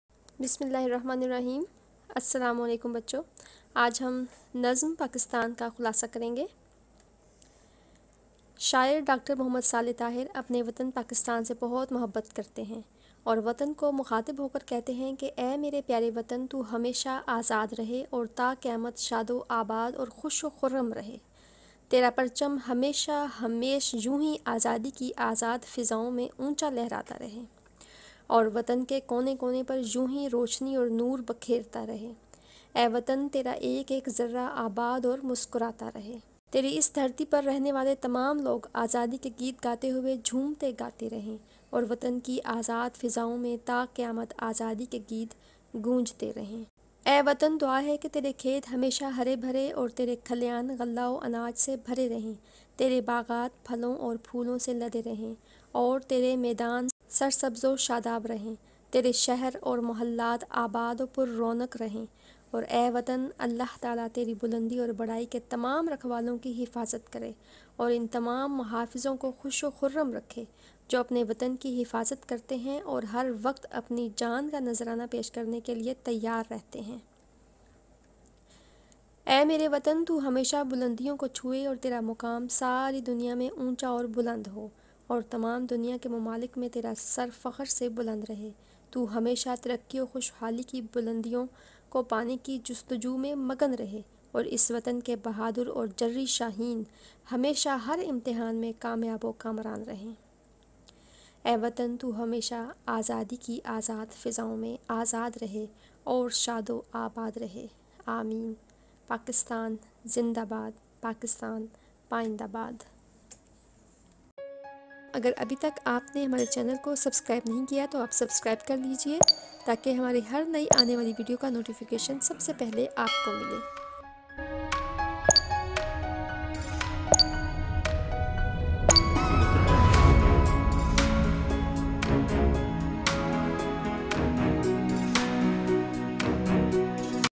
Pakistan(Khulasa) Nazm Chapter 25 Urdu Punjab Class 6
Dear Students, this is khulasa of nazm Pakistan
Also watch full Urdu playlist, LINK IS BELOW;